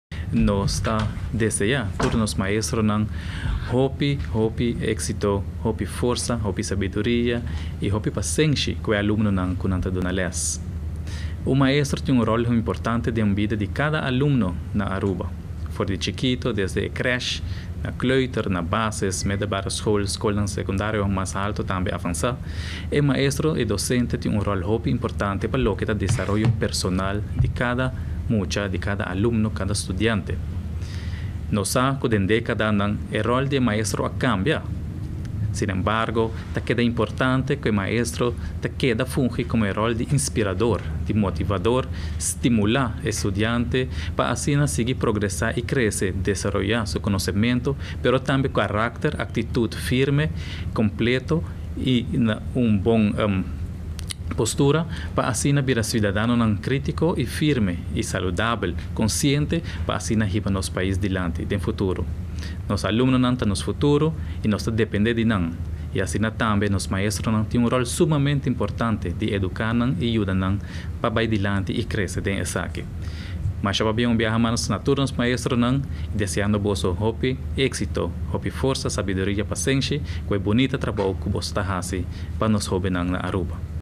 Nos docentenan ta personanan esencial den bida di cada mucha y hoben
Parlamentario Ricky Hoek a para un rato keto riba dia internacional di maestro. Tur docente ta importante pa cada mucha. E formación di un mucha ta depende hopi di su mayornan, pero e docente tambe ta un persona esencial pa e mucha y of hoben cu ta studia. sigui nos